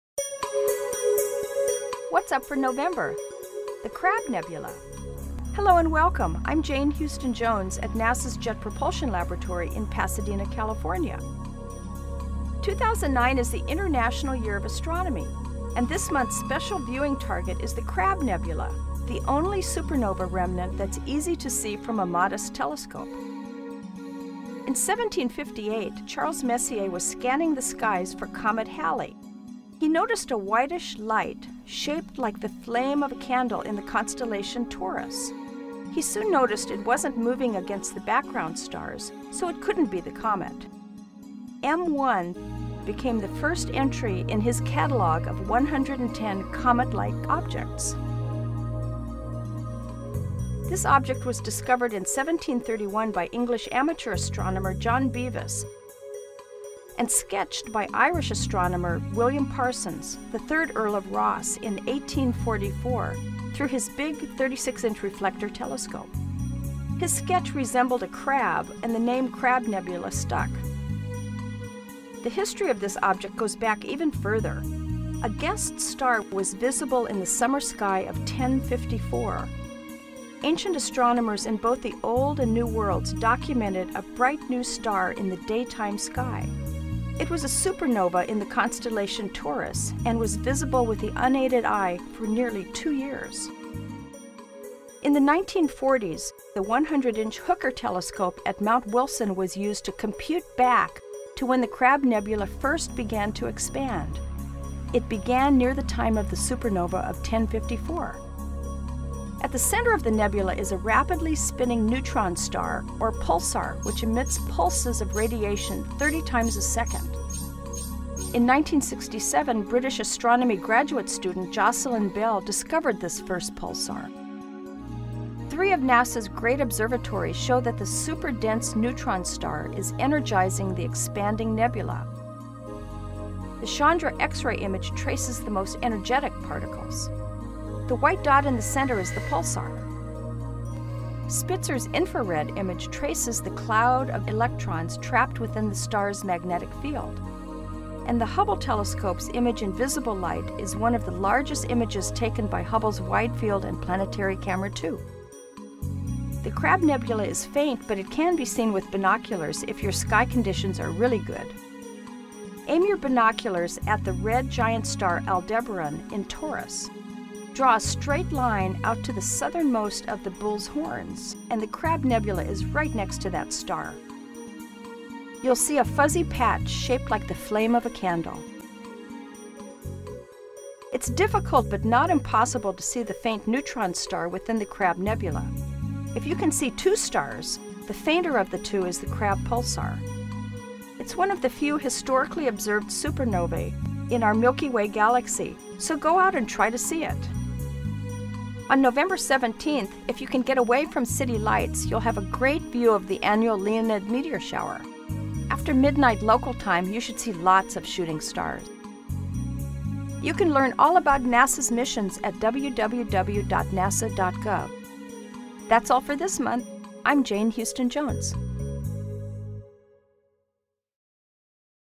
What's Up For Nov. 2009? Crab Nebula
This month you can see the remains of  a stellar explosion visible in 1054 - the Crab Nebula. And the Leonid meteor shower is on the 17th